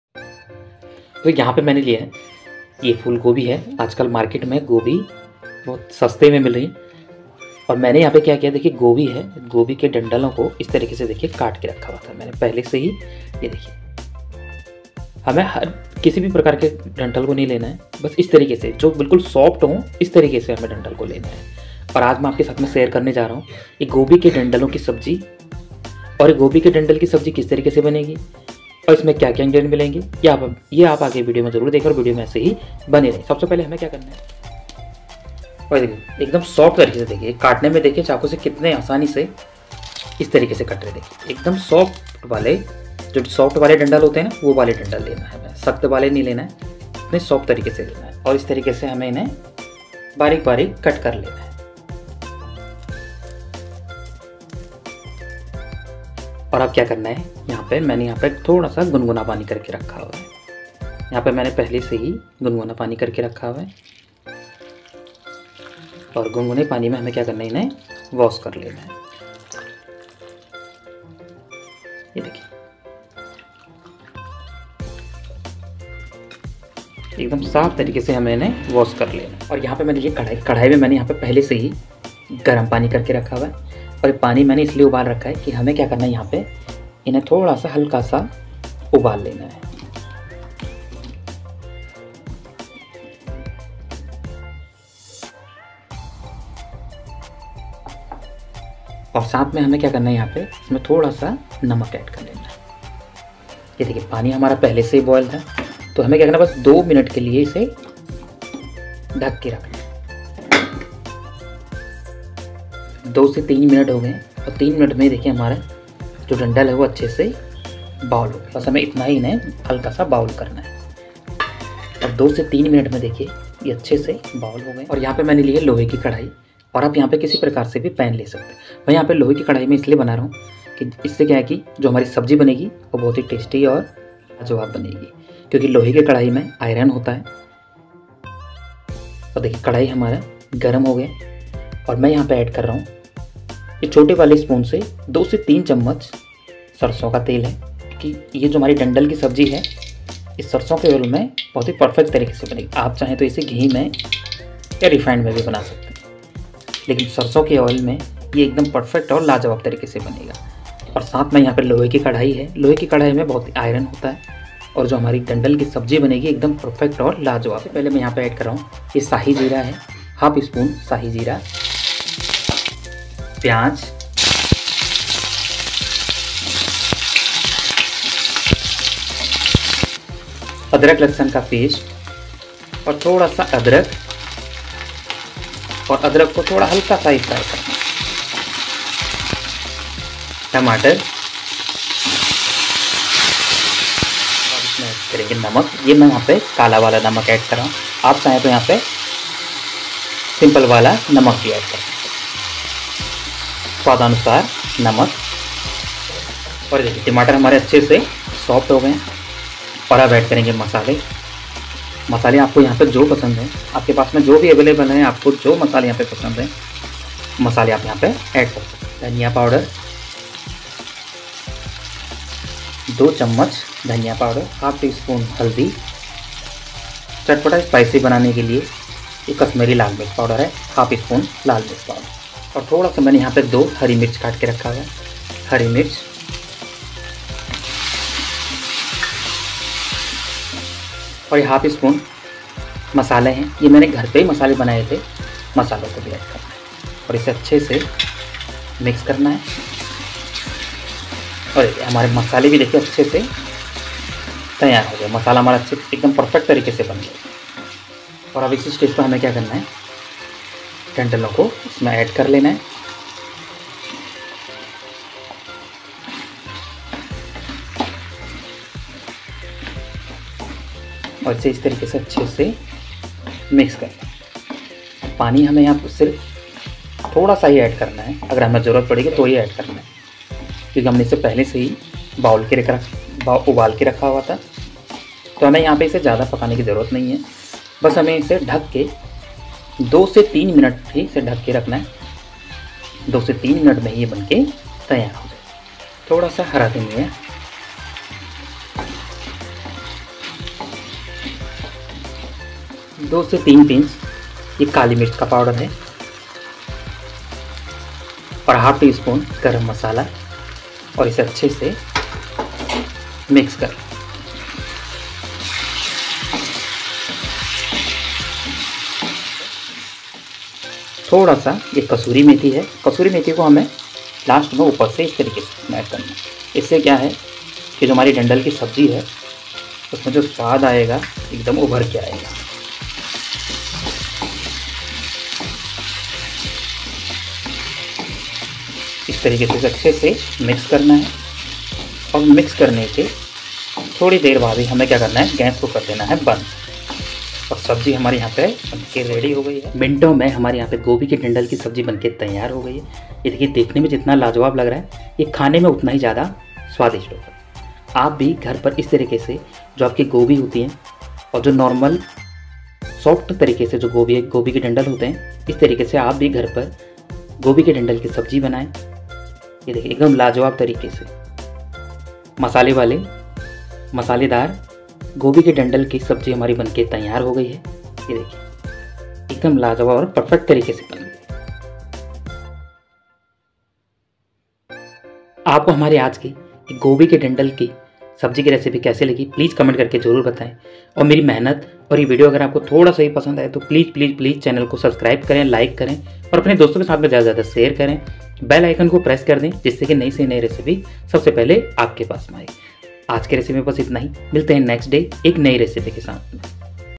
सोचा नही था कि इतनी टेस्टी और लाजवाब 😱 डंठल की सब्जी बनेगी | एक बार आप भी ट्राय करें गारंटी से 😋
सोचा नही था कि इतनी टेस्टी और लाजवाब 😱 डंठल की सब्जी बनेगी | एक बार आप भी ट्राय करें गारंटी से 😋 | @dayaljikadhaba  |

hello 👋 My YouTube Family Members कैसे है आप सब i hope आप सब स्वस्थ होंगे और मस्त होंगे तो दोस्तो आज मैं आपके लिए लेकर आया हूं घर पर रोज बनने वाली सब्जी नहीं सब्जी के डंठल की सब्जी जी हा दोस्तो सॉफ्ट वाले गोभी के डंठल की सब्जी रेसिपी व्यंजन विधि सामग्री अगर आपको मेरी मेहनत और लगन और वीडियो थोड़ा सा भी पसंद आए तो Please 🙏 Subscribe 🥺 Saport करना बिलकुल मत भूले

INGREDIENTS -
1/ GOBHI DANTHALE
2/ ONION
3/ TOMATO 🍅
4/ MUSTARD OIL 3 TSP
5/ SALT
6/ GREEN CORIANDER
7/ GREEN CHILLI
8/ CORIANDER POWDER - 2 TSP
9/ RED CHILLI POWDER - 1/2 TSP
10/ TURMERIC - 1/2 TSP
11/ GARAM MASALA - 1/2 TSP
12/ HOME MADE MASALA - 1 TSP